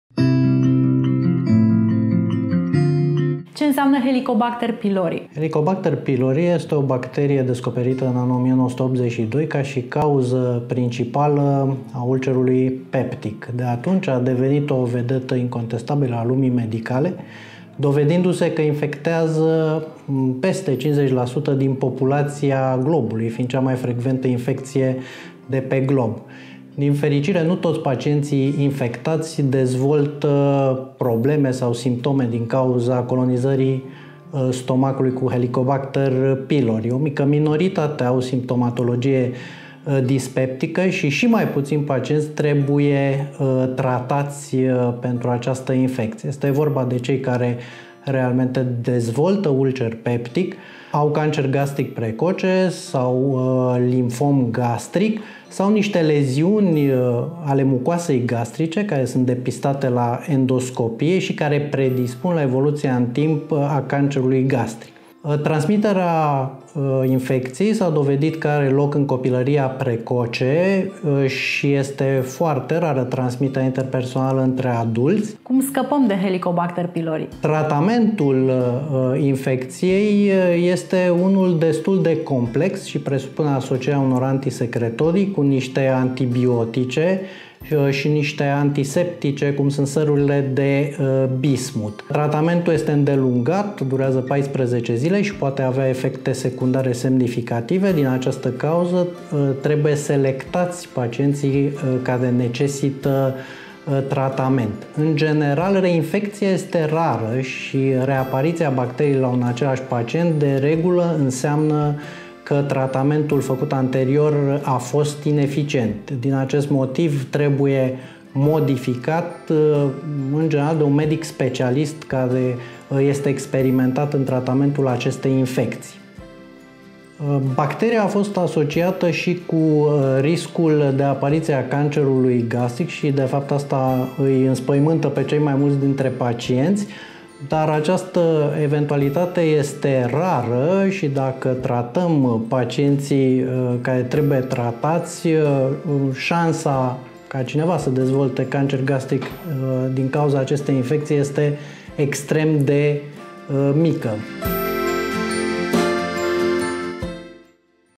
Dr Tudor Arbanas: Helicobacter Pylori
“Tratamentul pentru infecția cu Helicobacter Pylori este unul destul de complex și presupune asocierea unor antisecretorii cu antibiotice și antiseptice, cum sunt sărurile de bismut. Tratamentul este îndelungat, durează 14 zile și poate avea efecte secundare semnificative. Din această cauză, trebuie selectați pacienții care necesită tratament. În general, reinfectarea este rară”, ne spune Dr. Tudor Arbănaș, medic primar gastroenterolog.